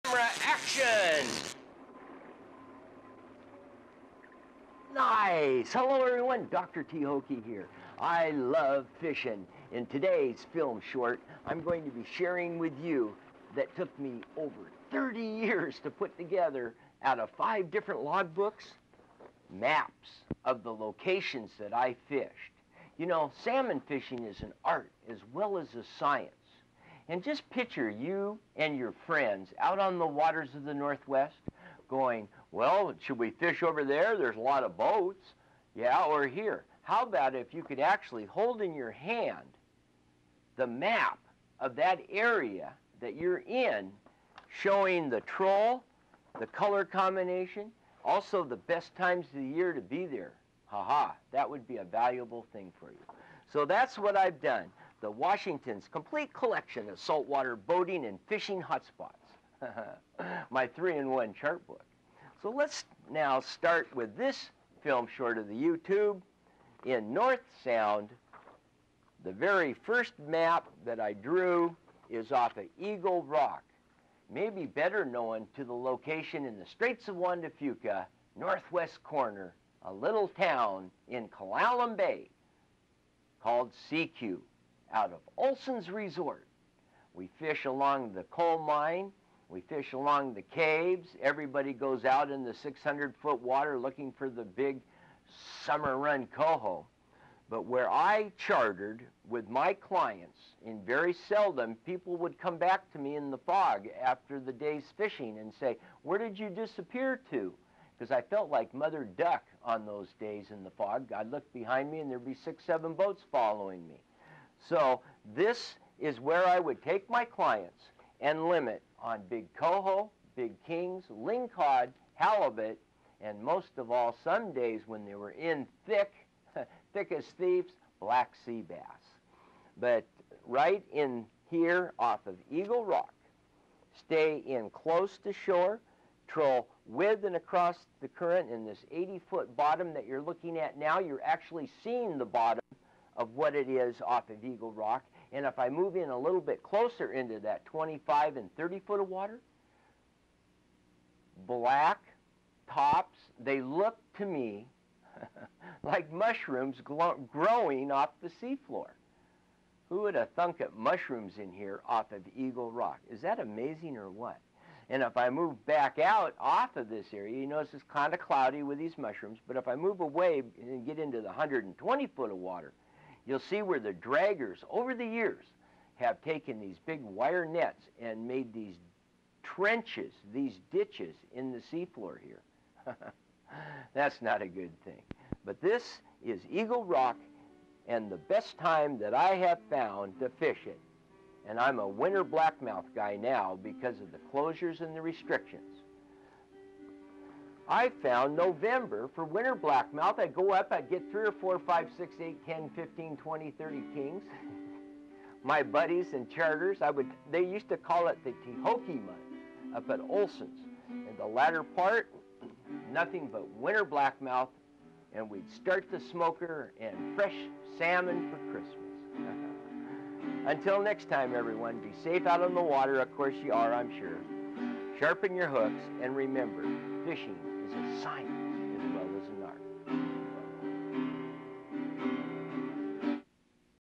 Sekiu Washington
Over the past 70 years Sekiu has been the sport salmon fishing place to fish. SEKIU means in tribal tong "Quite water" They say if you want to catch salmon fish were the salmon are. Remember, trolling for salmon is a science as well as an art. "I know this info will improve your salmon catch".        Please, let me know any questions you have.  Your friend Doc Thoke.